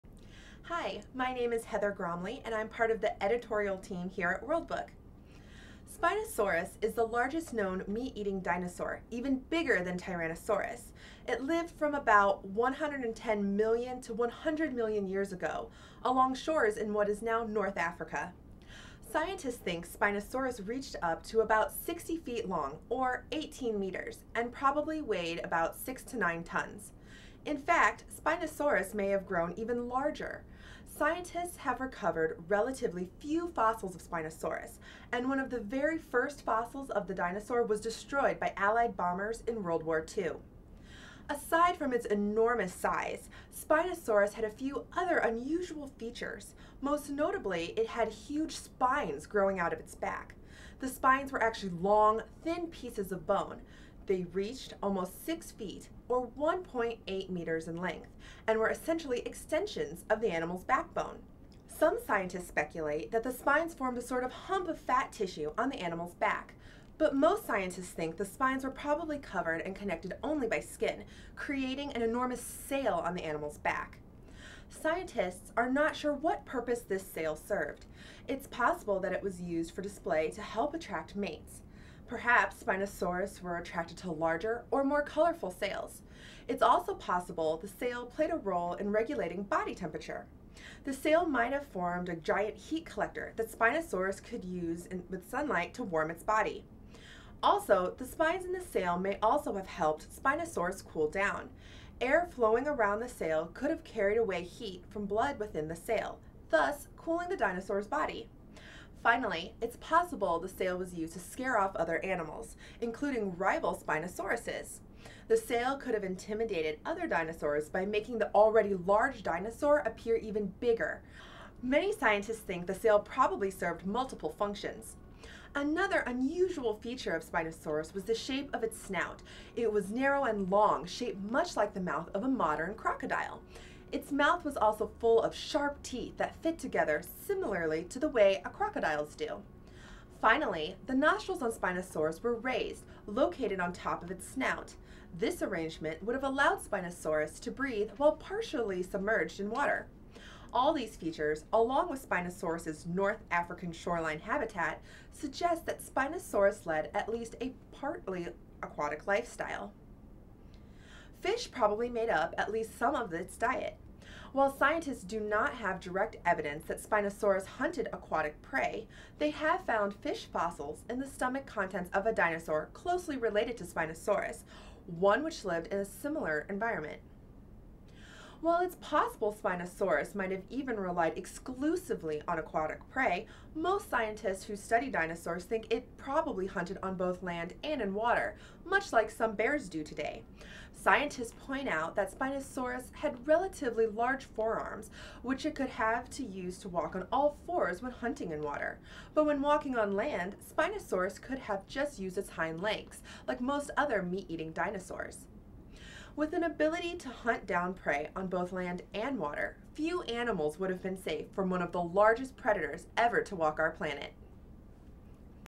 What was the purpose of Spinosaurus' sail?
World Book's Heather Gromley explains the purpose of Spinosaurus' snail.

Spinosaurus is the largest known meat-eating dinosaur, even bigger than Tyrannosaurus. It lived from about 110 million to 100 million years ago, along shores in what is now North Africa.

This video was created by World Book Encyclopedia.